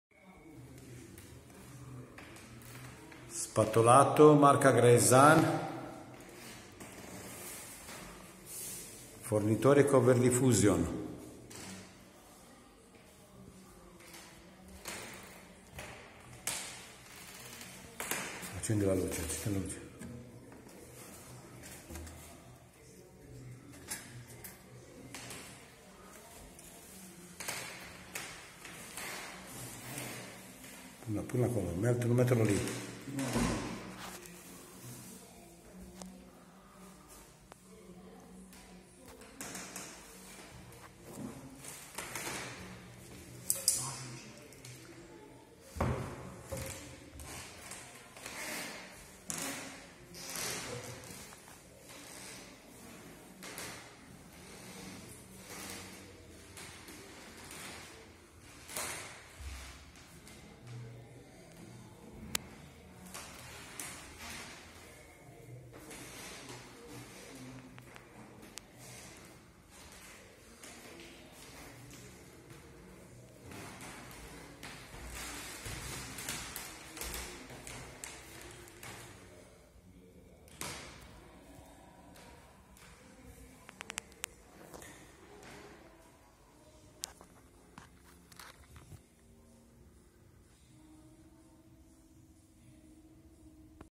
Spatulato marca Graesan Fornitore Coverlifusion
this is a venitin stucco created by the famous decorator Gioirgio Graesan